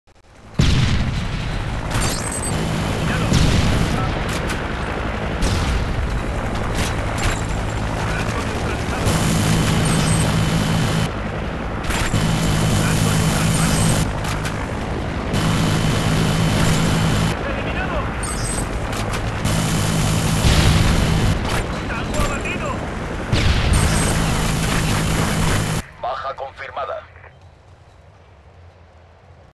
LA MODA DE CONSEGUIR LA MOAB - MaHzUz55
Pues otros dos artistas consiguiendo la MOAB, uno pone la cabeza y el otro la bala, evidentemente el que consigue la MOAB no es el que pone la cabeza, en este caso kzR x l3g10N (LEGION) hace la MOAB y KzR x R4P1DzZ (RAPID EN MORIR JE JE JE) pone la cabeza. Un saludo majos y a hacer más MOAB.